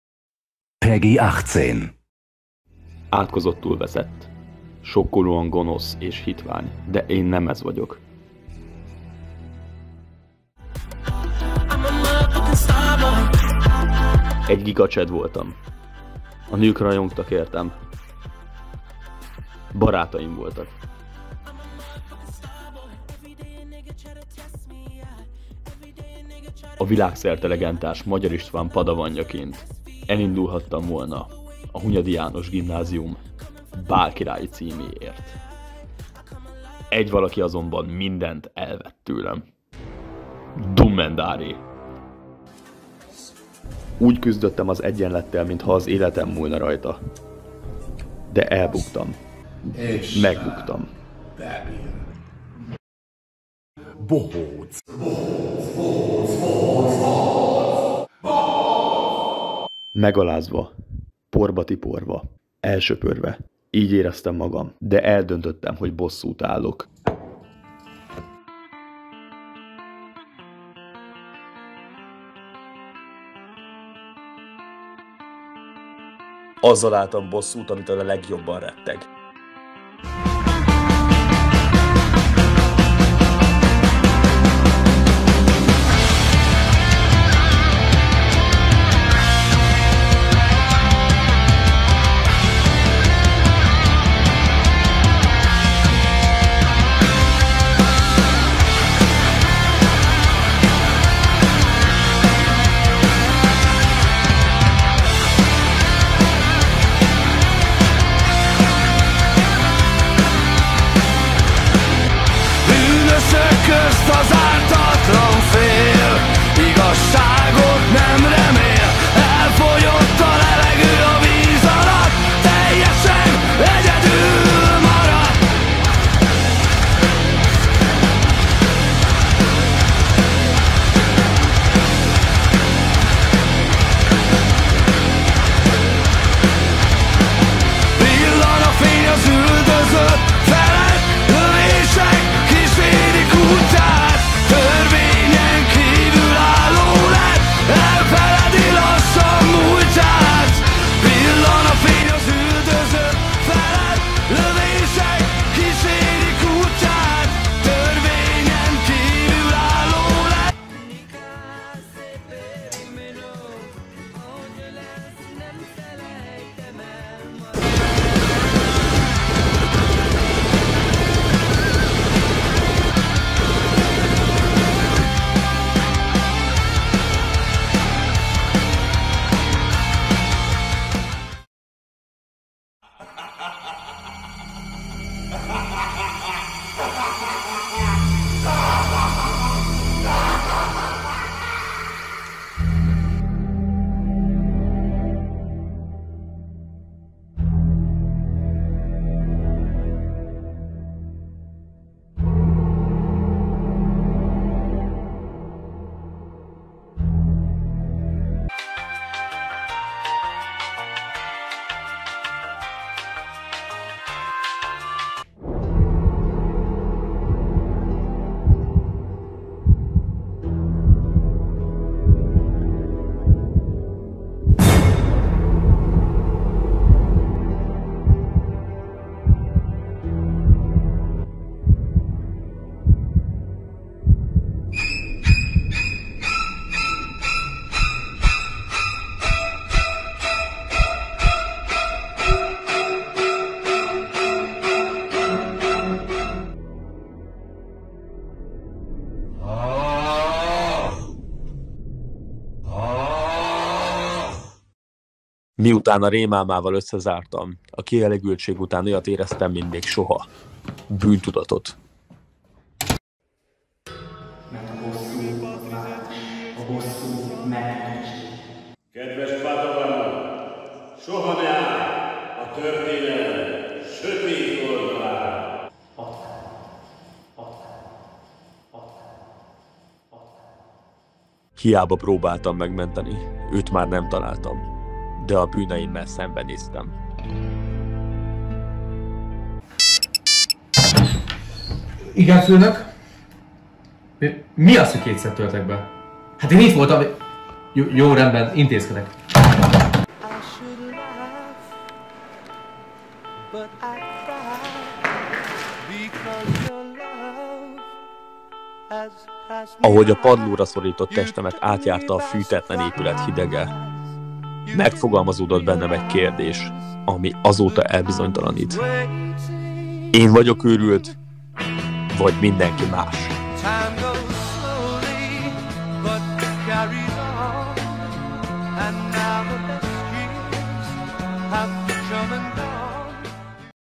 Veres Marcell kampányfilm * Hunyadi 2023
XVII. Hunyadi Bálkirály & Bálkirálynő Választás 2023
Veres Marcell bálkirály jelölt kampányfilmje.
A jelöltekre a Hunyadi János Gimnázium hivatalos facebook oldalán lehet.
Eredményhirdetés a Hunyadi János Gimnázium SZALAGAVATÓJÁN, a "Végzősök Bálján", 2023. március 4-én.
Mezőkovácsházi Hunyadi János Általános Iskola, Gimnázium és Kollégium